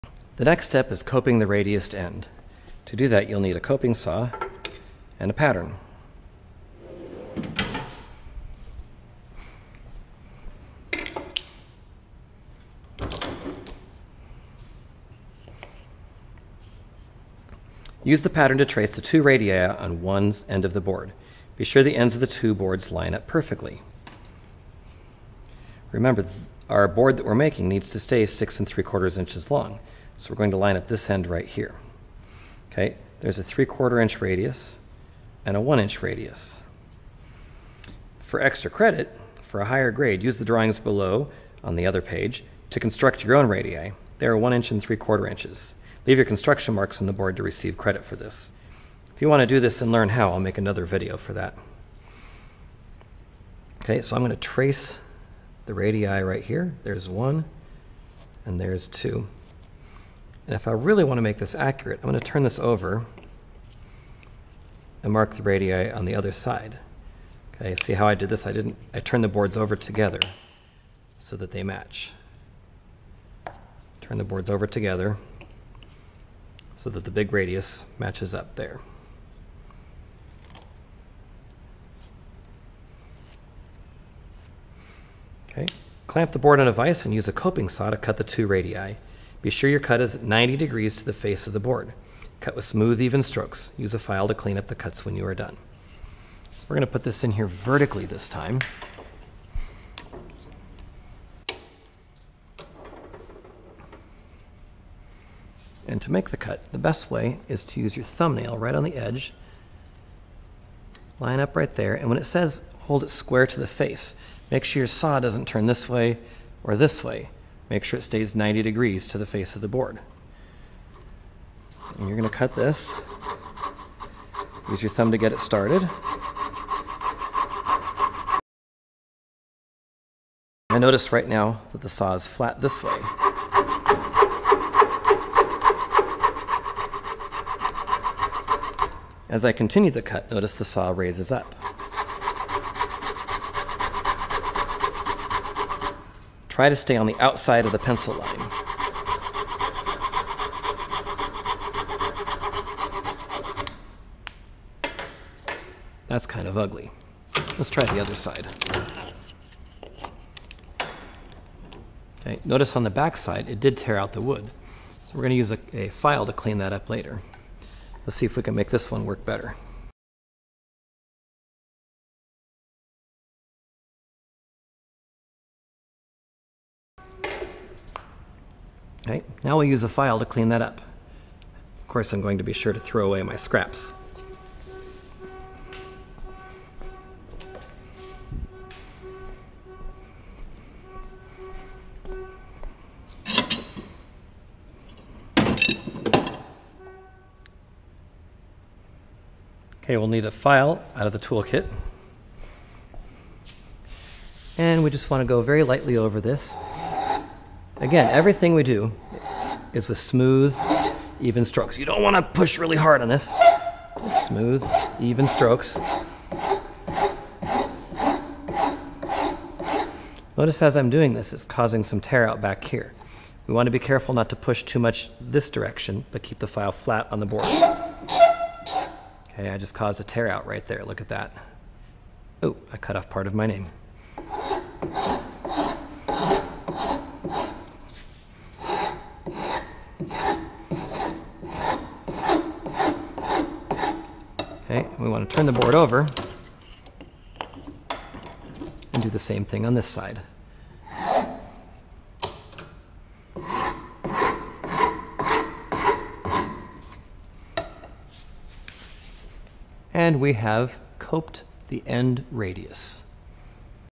Hand Tool Project Cut End Radii
This is part 4 of the Hand Tool Project.  I will show you how to use a coping saw to cut an end radius.